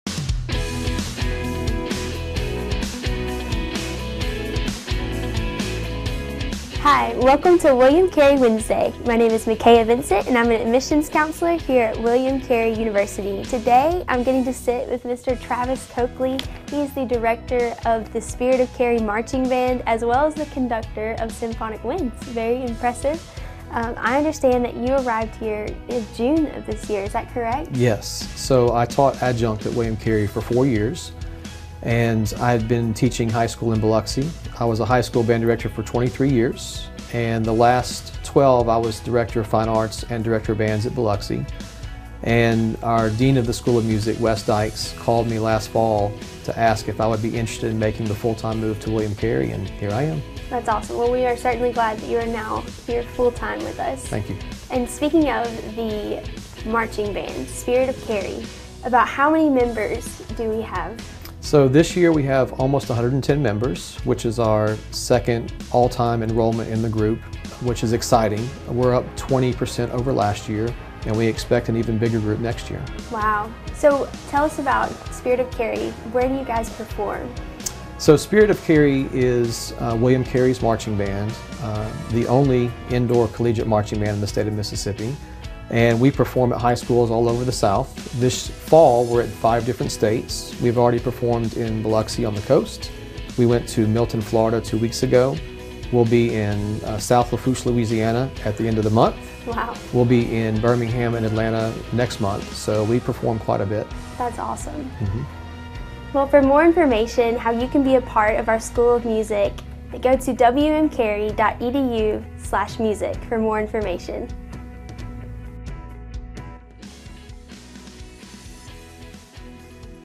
William Carey Wednesday - Spirt of Carey
This week on "William Carey Wednesday," we sit down with Mr. Travis Coakley, the director of the Spirit of Carey Marching Band! Discover more about the only collegiate indoor marching band in the state of Mississippi. Interested in the William Carey University School of Music?